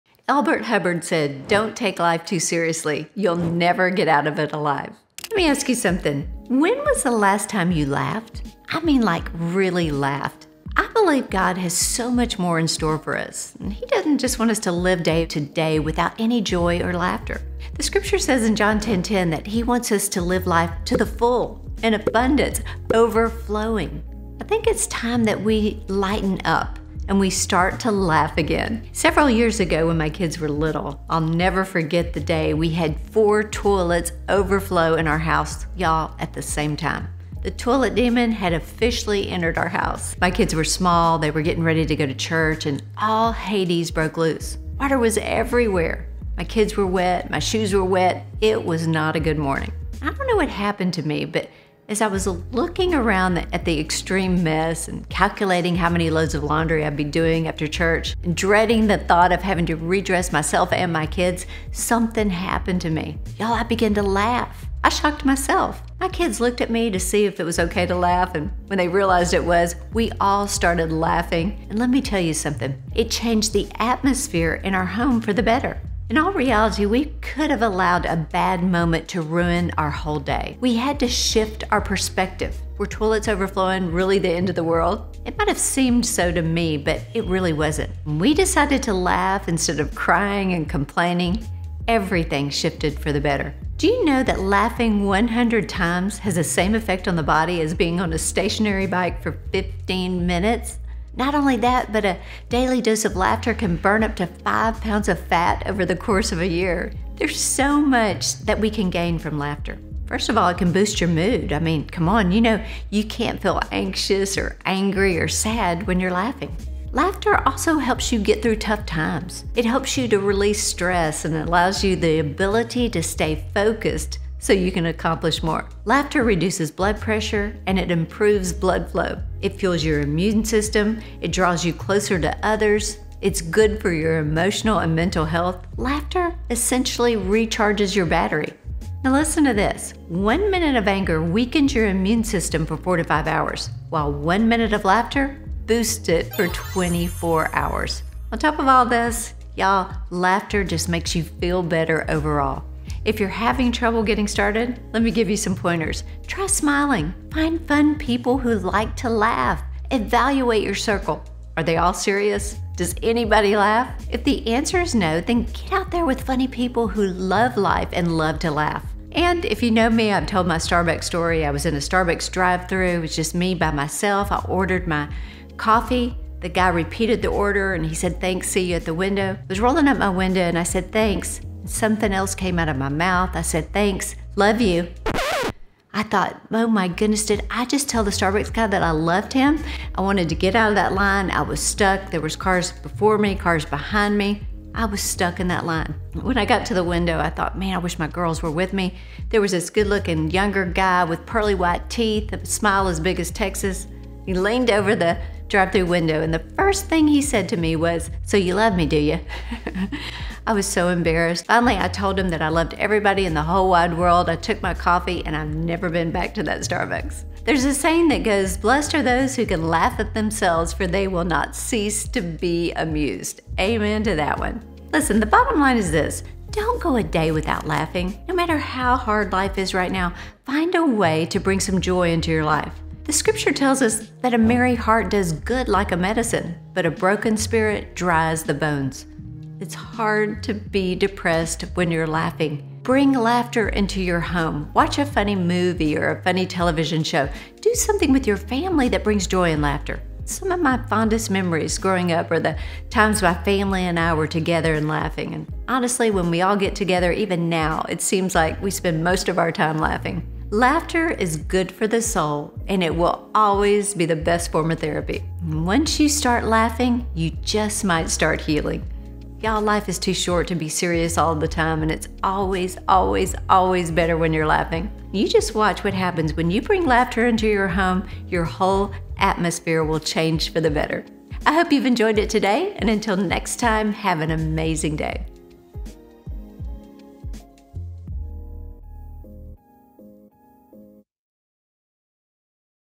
Get Some Joy Back In Your Life! | Laughter is ALWAYS the Best Medicine | April Osteen Simons
Life is serious, but never too serious to take time to laugh.It's amazing what laughter can do for the soul and the mind. Need a boost in life? A daily dose of laughter can change everything for the better.